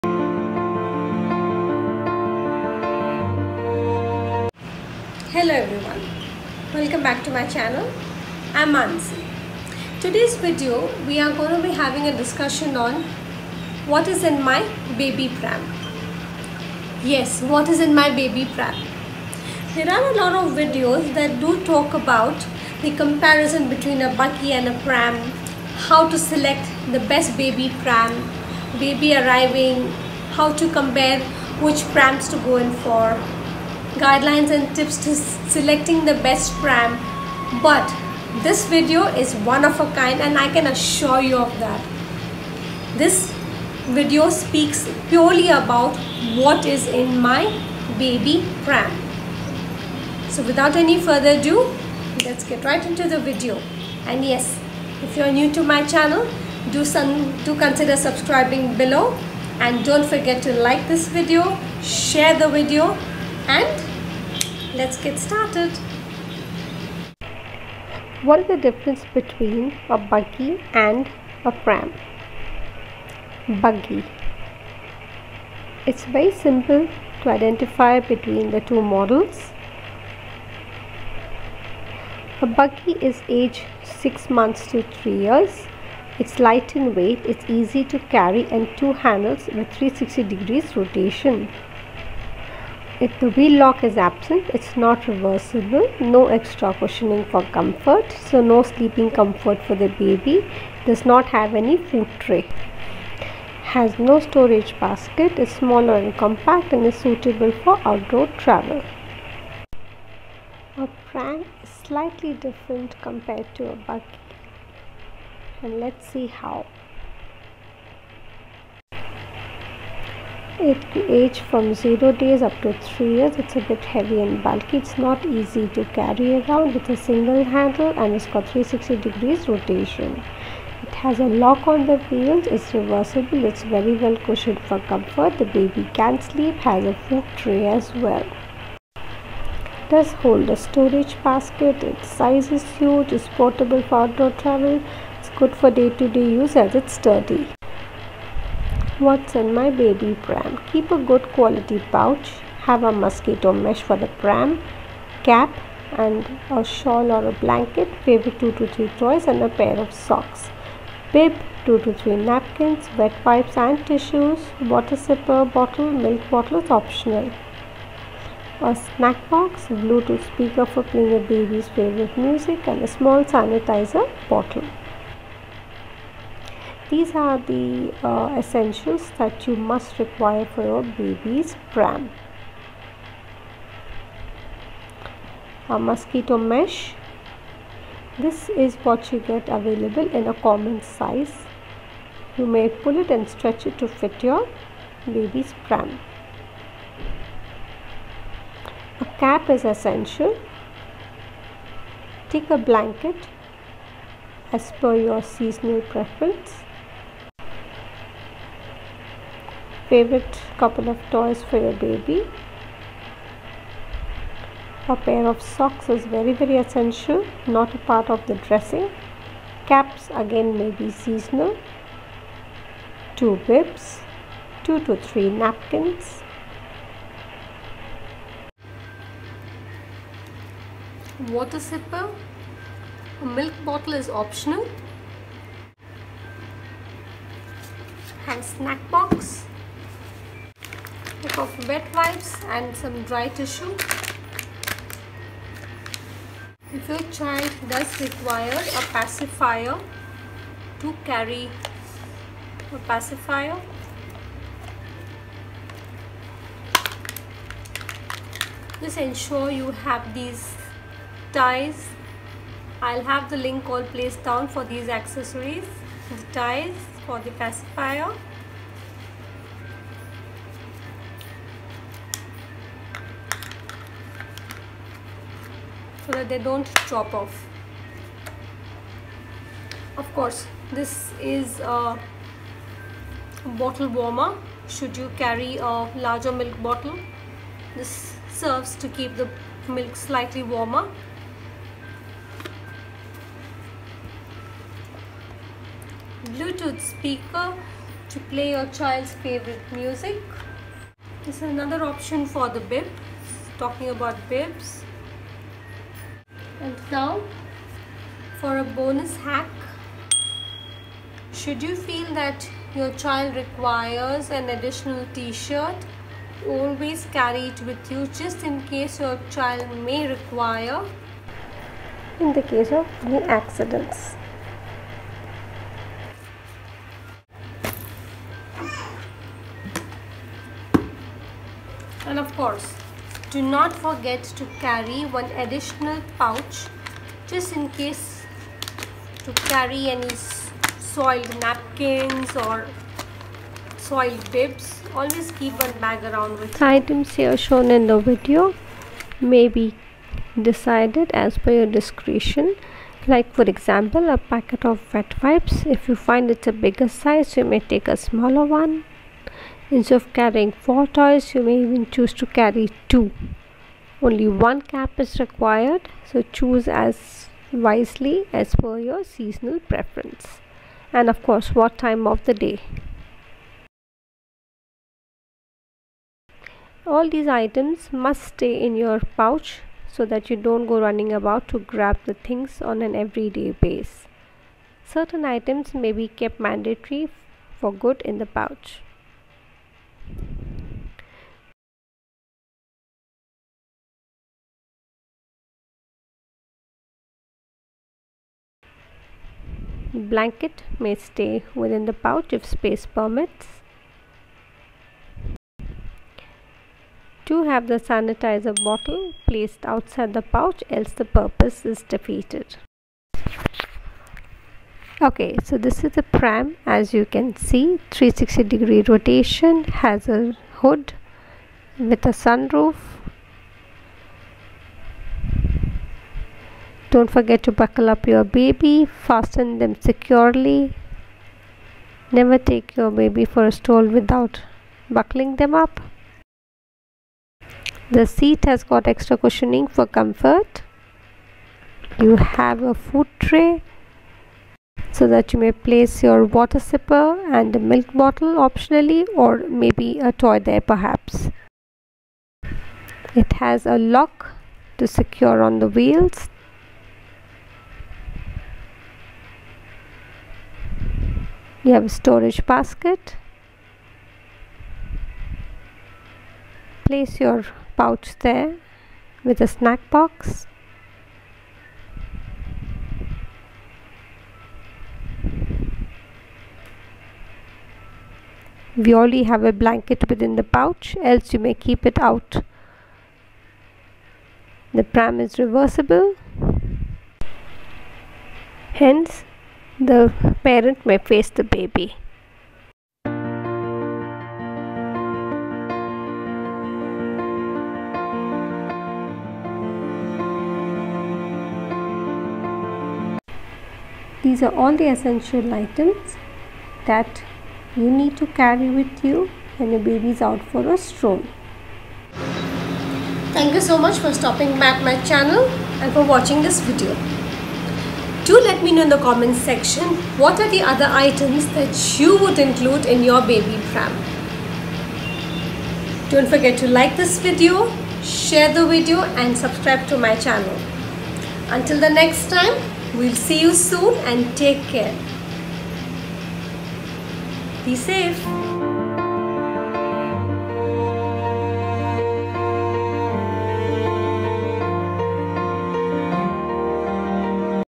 What’s In Your Baby’s Pram ? This is in my Baby’s Pram !!!
This video brings to you the one of a kind information, which no one has ever spoken about nor shared
“ what’s in your baby’s pram “

All videos show how to compare and choose the pram , or compare Buggie vs Pram

Here your questions are answered  !

Mesh : Generic Baby Carriage Stroller...

Pacifier : Chicco Physio Comfort Soother (Pink)

Baby Teether Holder

Milk Bottle 🍼

Water Sipper : Philips Avent Classic Soft Spout...

Thanx 🙏🏻